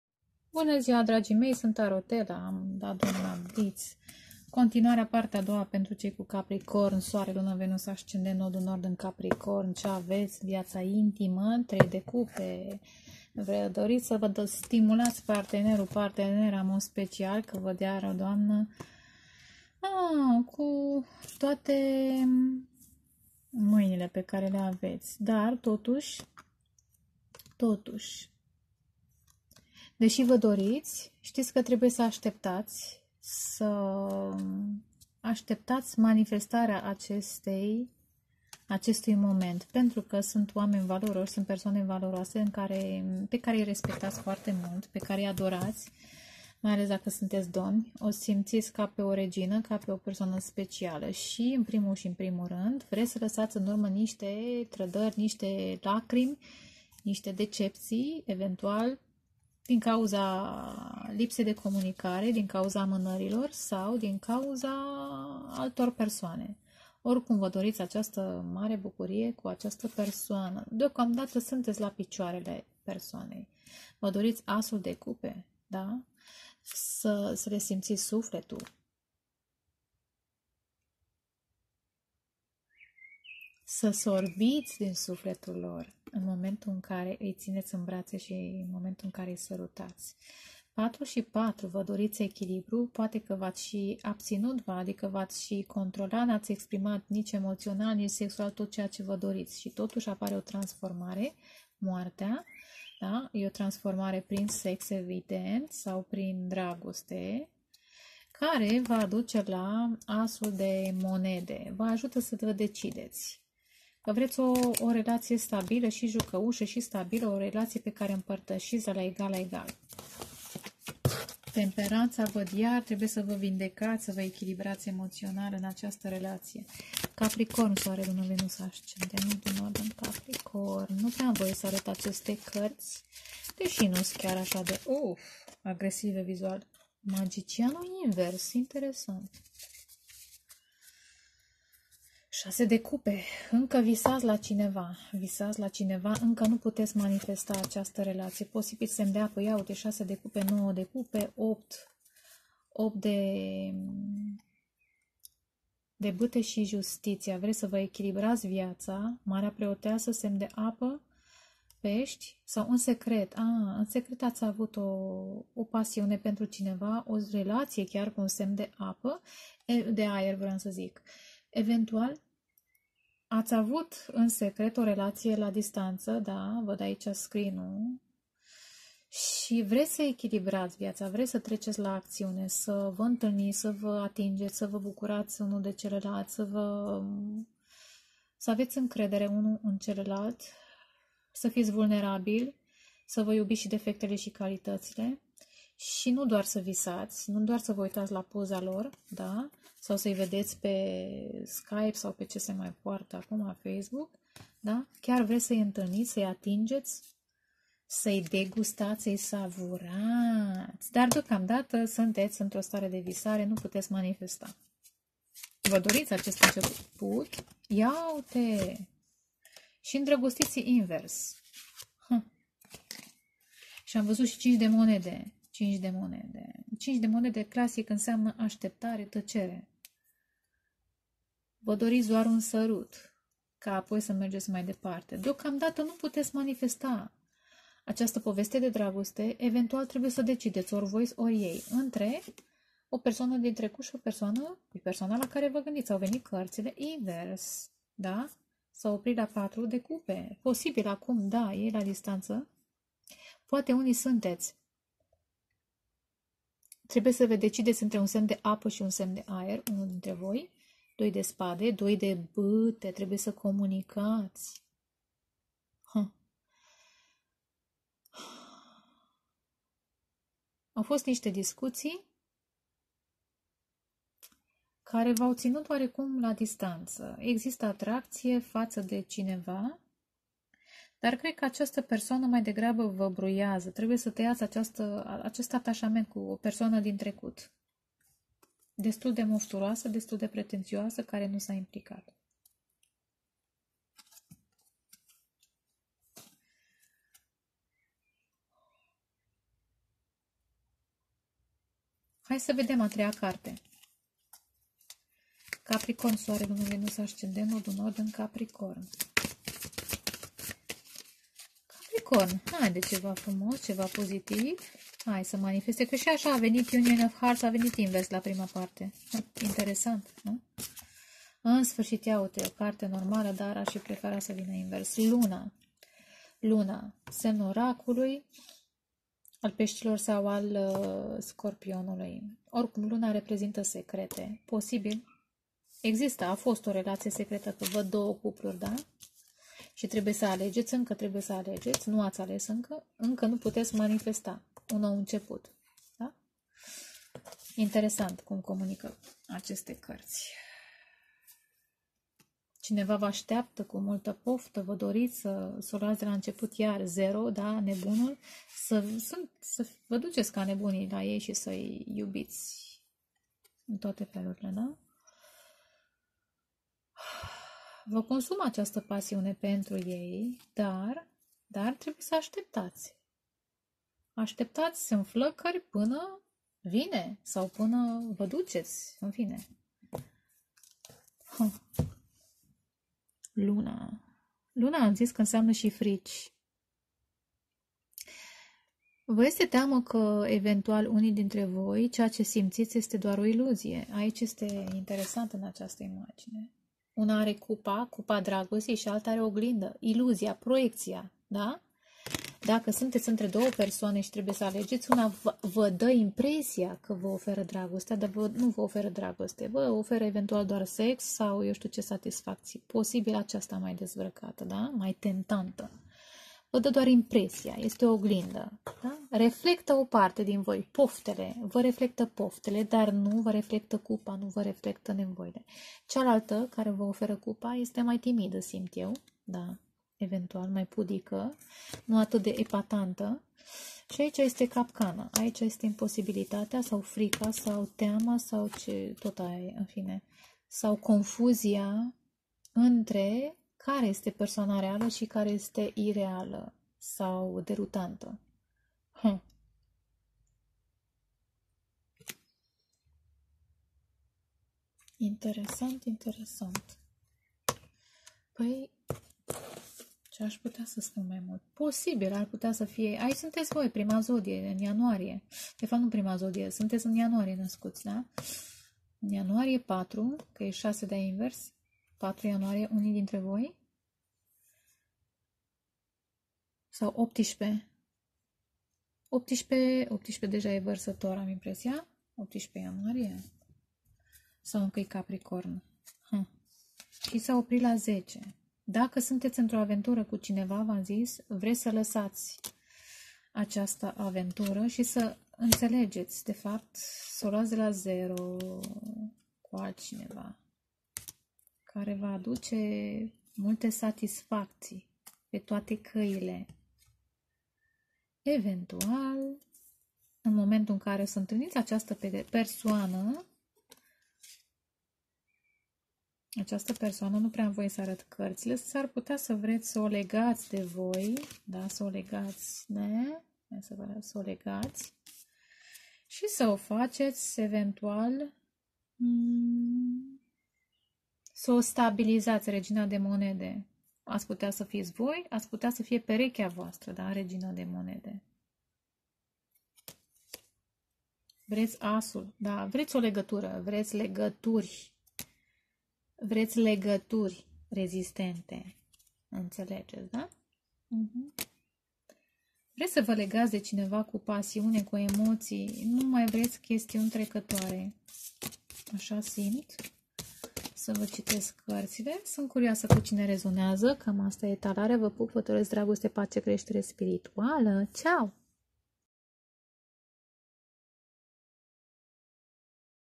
CAPRICORN - IUNIE 2020 - PARTEA A 2 A
Etalare pentru zodia Capricornului: Soare, Luna, Ascendent, Venus sau Nodul Nord. A se retine ca aceste citiri sunt colective. Va invit sa luati informatiile care corespund vietii voastre particulare.
Ma puteti urmari si pe facebook.
Va astept. Multumesc.